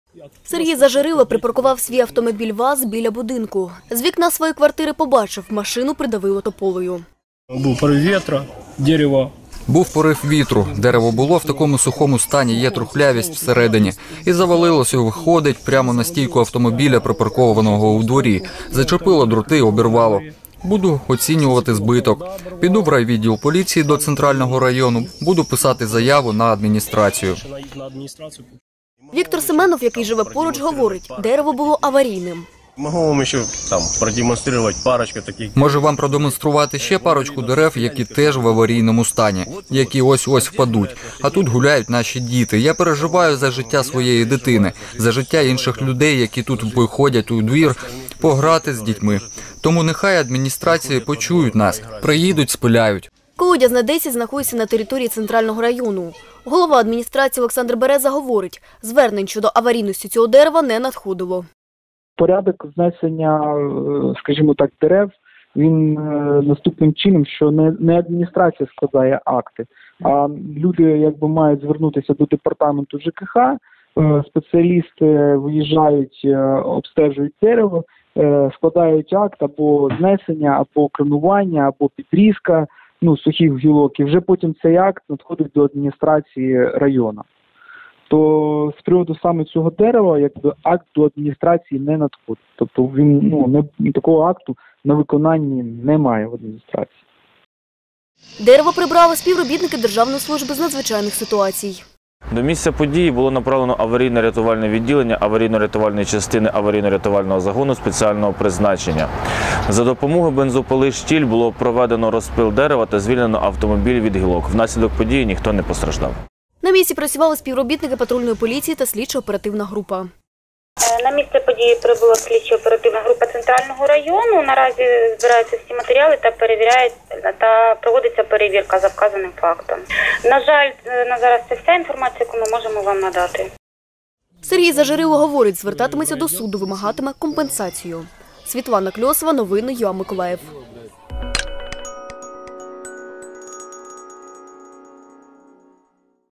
У Центральному районі на автомобіль впало дерево
Через негоду на вулиці Колодязній, 10, міста Миколаєва, дерево впало на автомобіль.
Відео Ігоря Арчибісова